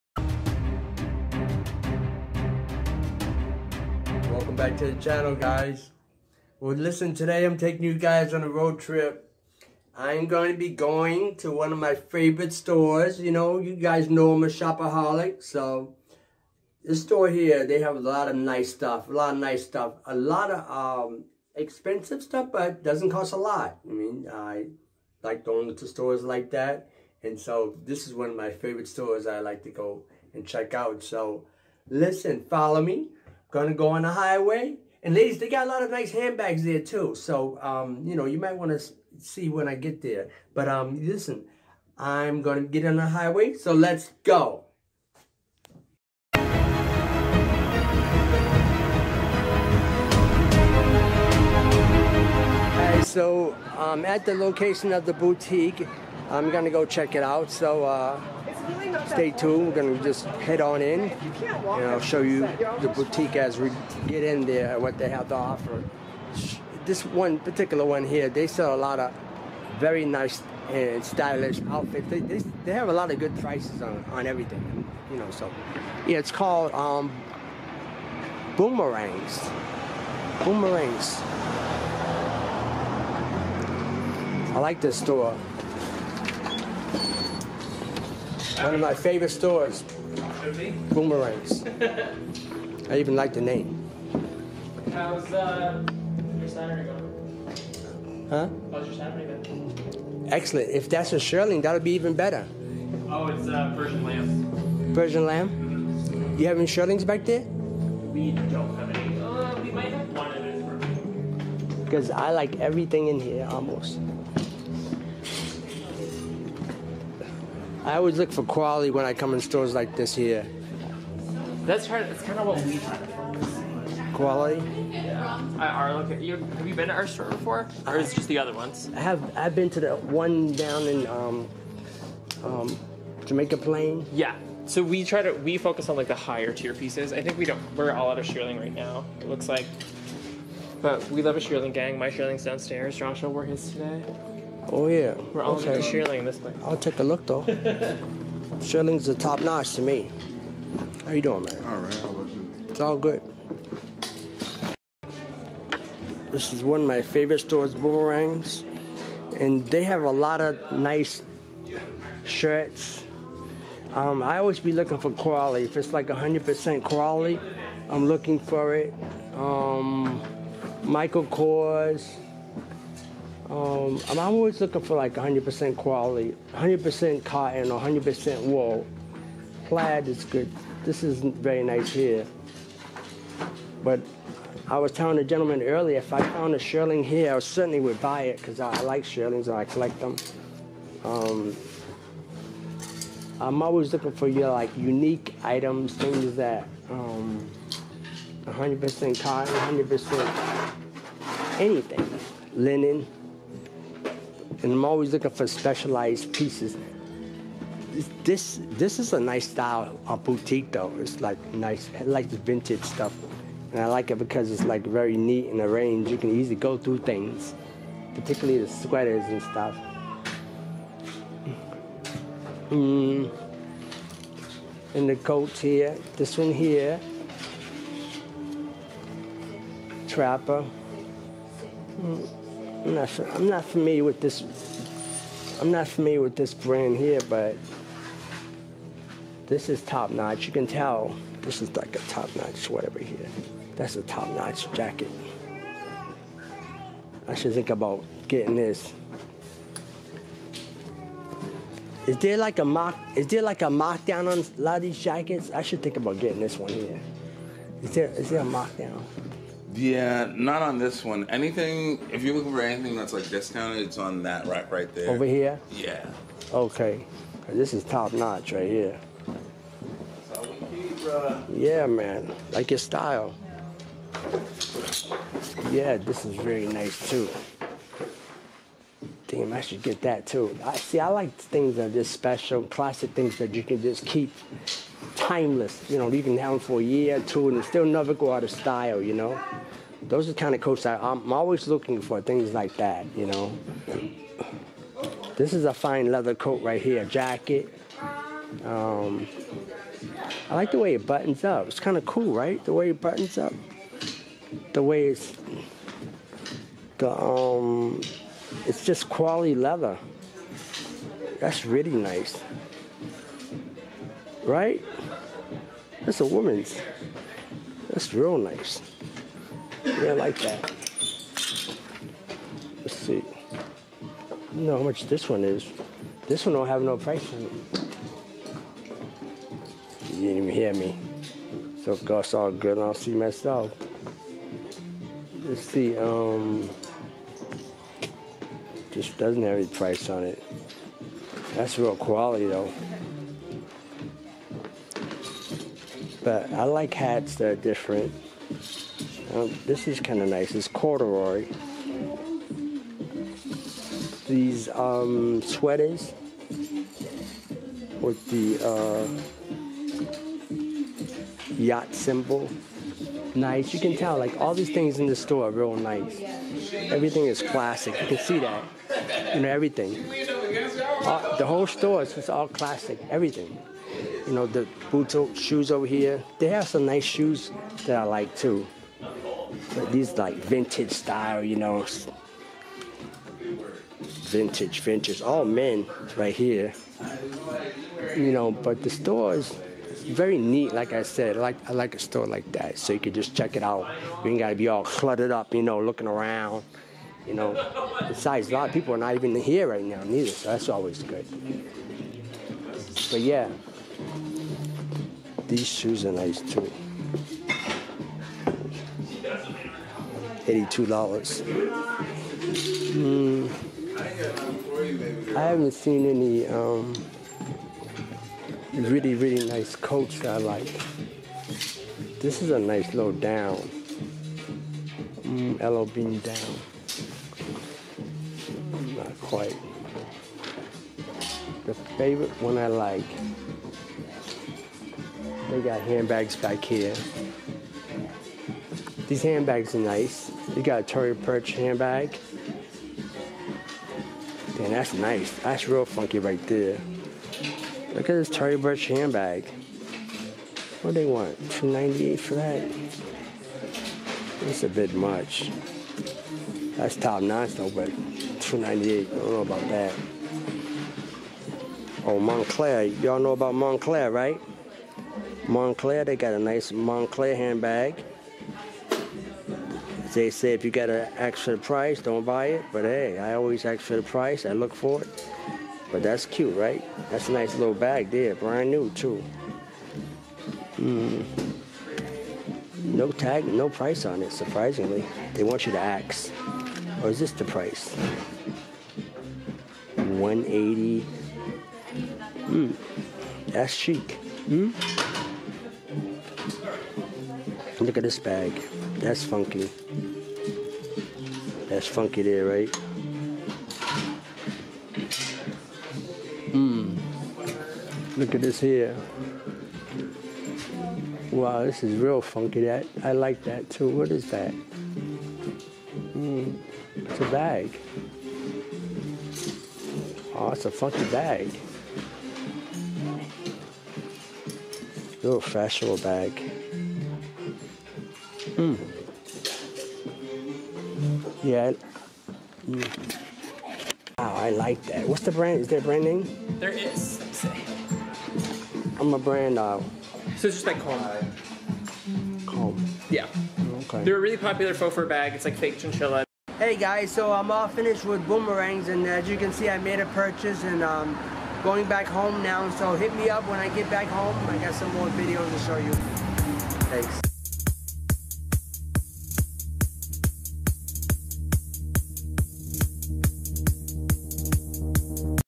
Damien's Designer Clothing Shopping Vlog
Designer brands and vintage clothes. What type of clothing  do you like!
Damien takes you on a shopping trip to find a hidden gem. You can find exclusive stores like this in your city. Just look!